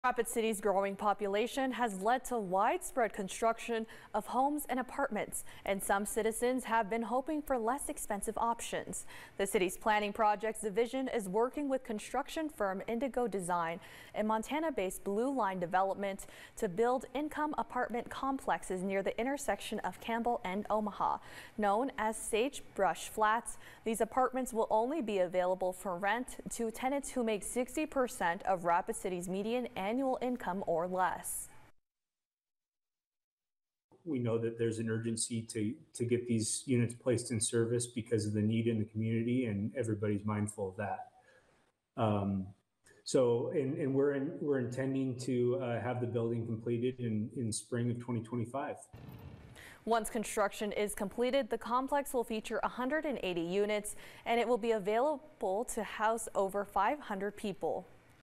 Rapid City offers more affordable housing options
Rapid City's growing population has led to widespread construction of homes and apartments.